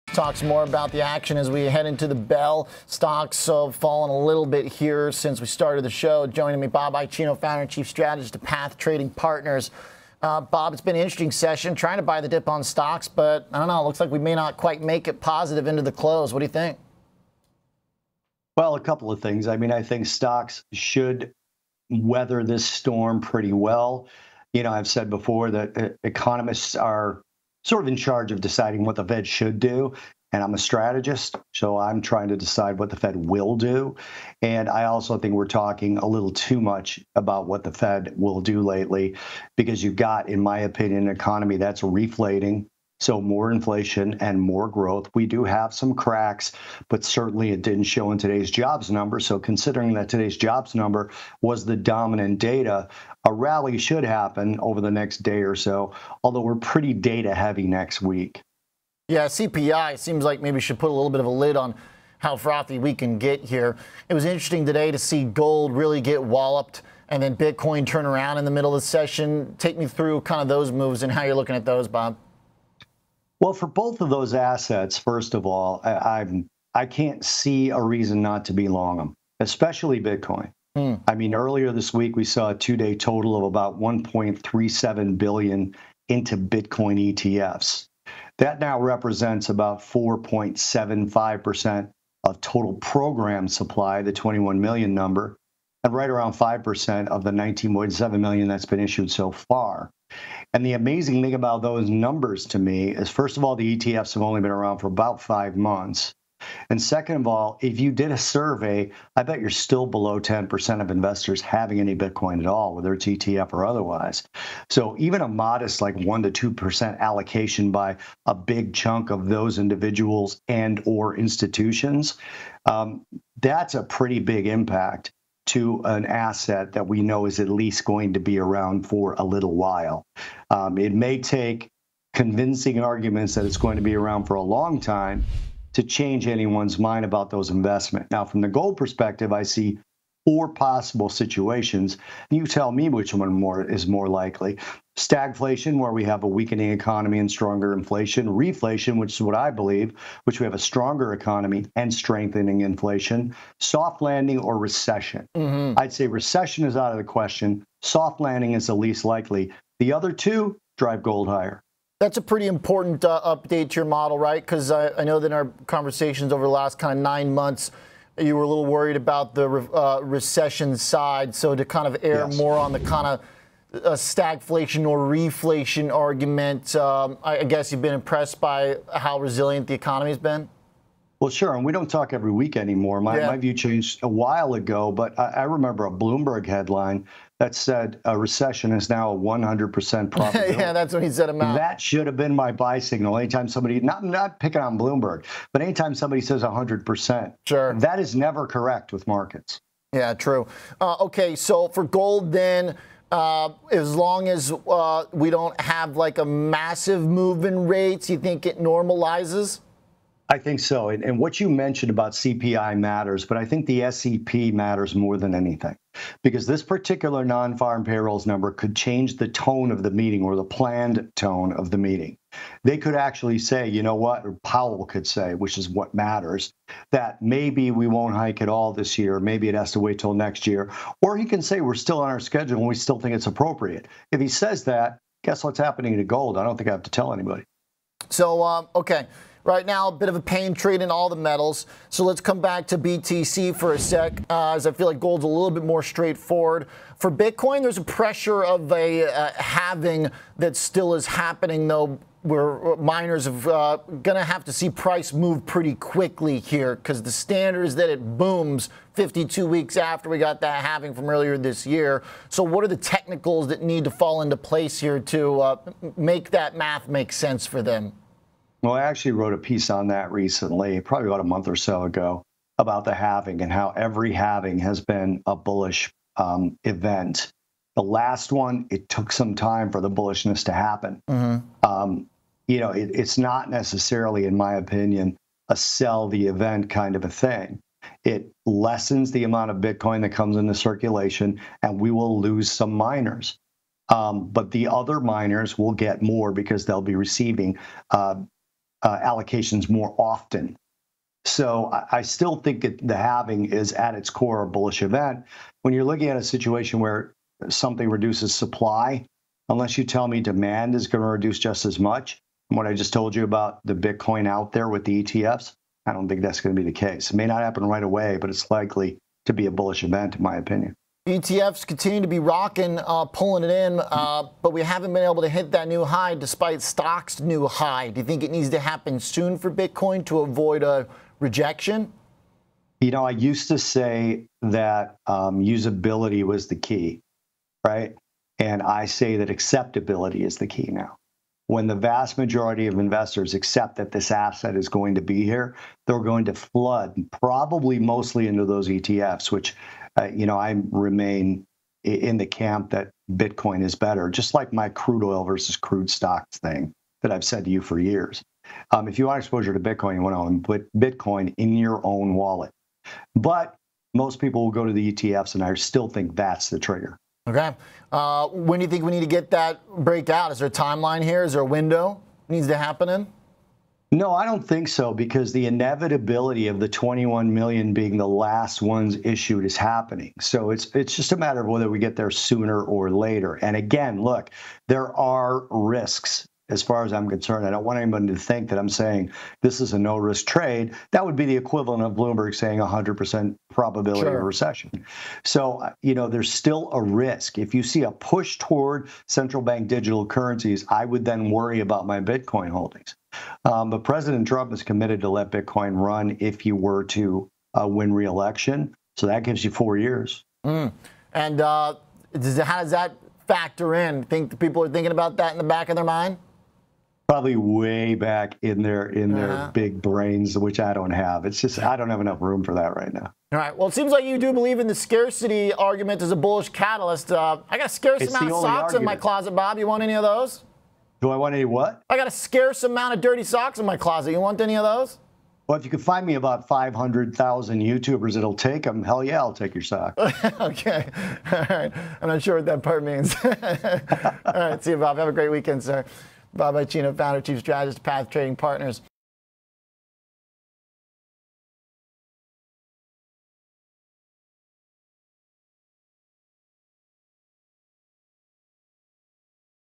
Levels to Watch in Gold & Potential Bitcoin Catalysts
Gold retreats below $2,400. Bob Iaccino discusses the outlook for gold (/GC) and Bitcoin (/BTC). He talks about levels to watch on gold futures, as well as potential Bitcoin catalysts. Tune in to find out more about the stock market today.